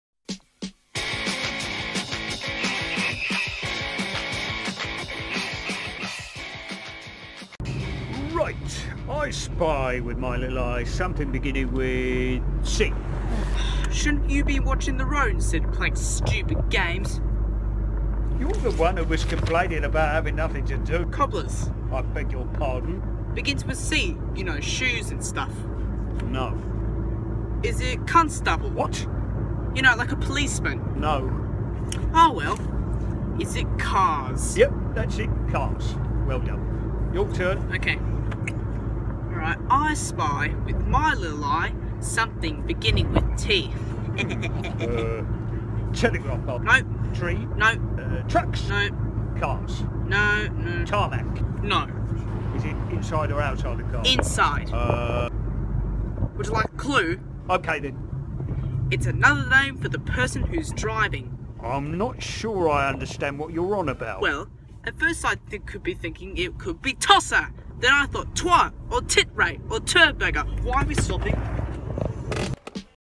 I Spy - Annoying Kid
I thought I’d spice up the in-car entertainment.